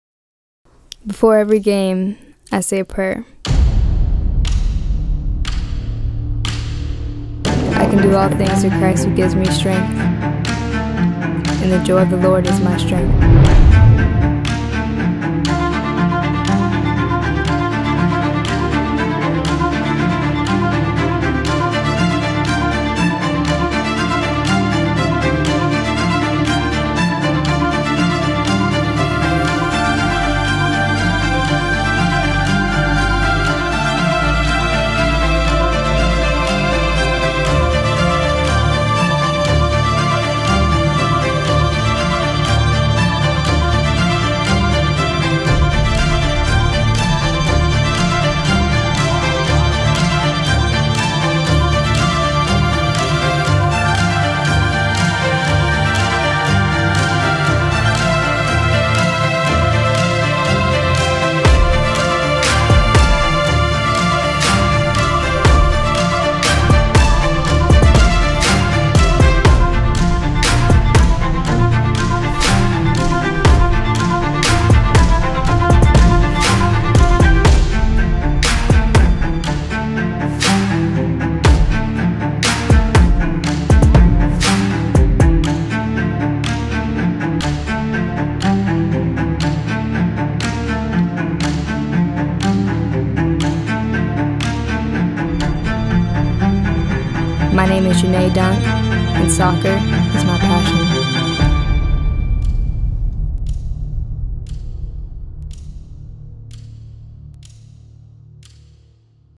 Janna Dunk Recruitment Video 2019
Janna Dunk's soccer recruitment video for college. All footage is real-time unless slowed down.

High School Career Stats:
40 Goals
20 Assists
2018 League MVP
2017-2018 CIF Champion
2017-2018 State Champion